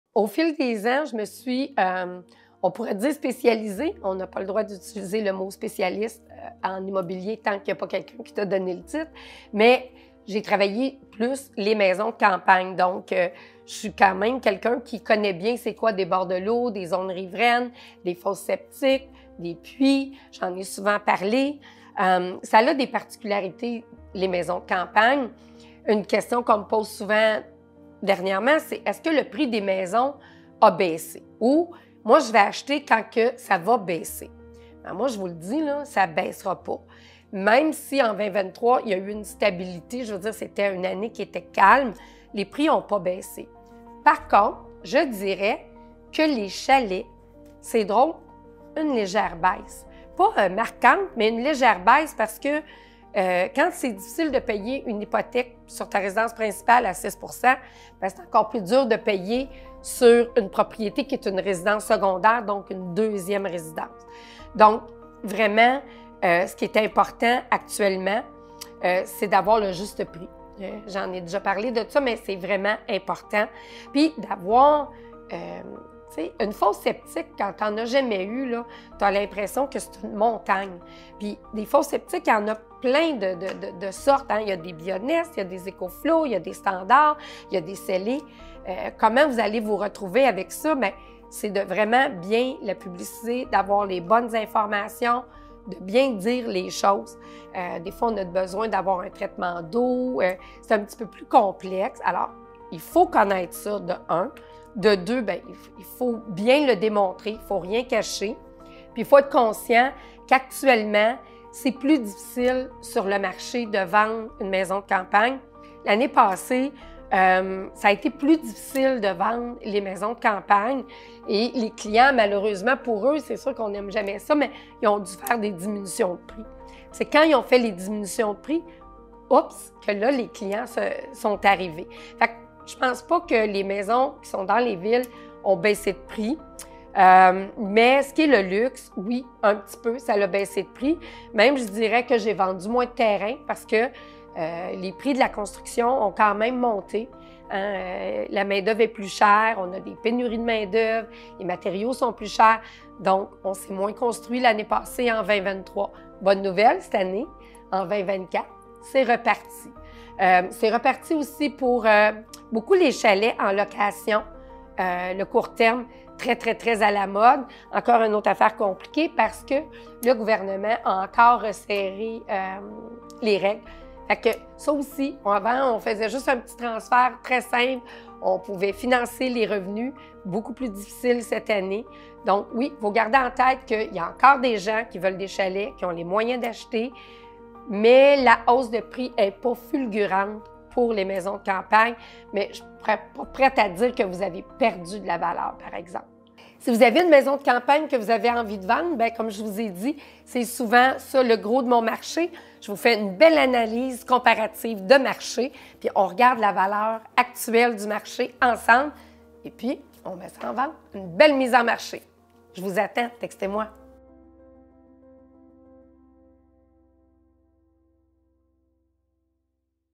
Les Maisons de Campagne Ma Signature dans l'Immobilier
Au fil des années, je me suis « spécialisée » dans la vente de maisons qui offrent non seulement un refuge paisible mais aussi des caractéristiques uniques 🌿: que ce soit un accès privilégié au bord de l’eau 🌊, des zones riveraines, ou encore les particularités techniques telles que les fosses septiques et les puits artésiens 💧.

Chaque propriété campagnarde a sa propre histoire et ses spécificités, et il est crucial de les comprendre pour faire un investissement avisé 📈.

Certains pensent que le marché des chalets est en baisse, mais la réalité est plus nuancée. Bien que le coût des prêts hypothécaires ait augmenté, rendant le financement plus complexe, le juste prix reste la clé.

2023 a vu moins de constructions et de transactions, mais 2024 a démarré en légère hausse, avec encore de nombreux acheteurs intéressés par un chalet ou une maison de campagne 🏡.

Il est essentiel de ne rien cacher et de bien expliquer les implications de ces caractéristiques uniques pour que vous puissiez prendre la meilleure décision 🌟. Si vous envisagez de vendre ou d’acheter une maison de campagne, n’hésitez pas à me contacter pour une analyse comparative du marché 📊.

Ensemble, nous pouvons réaliser une belle vente ou trouver la perle rare que vous recherchez.

Il me fera plaisir d’en discuter avec vous 💬.